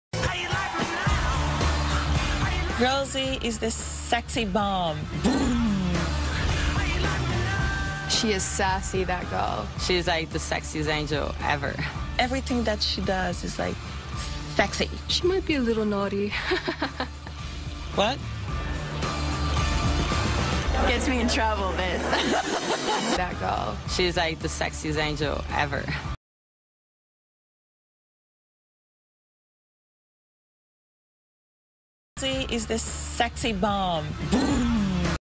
READ MY LIPS ROSIE HUNTINGTON WHITELEY OVER HER FAMOUS POUT/DUCKFACE 我愛台妹 - The Best Documentary Ev
READ MY LIPS ROSIE HUNTINGTON WHITELEY OVER HER FAMOUS POUT/DUCKFACE 我愛台妹 Victoria's Secret models give lip to Rosie .

(null)

Watch Behind the scenes as Alexi shoots Rosie Huntington-Whitely for Harper Bazaar UK! Video by Stephan Alessi.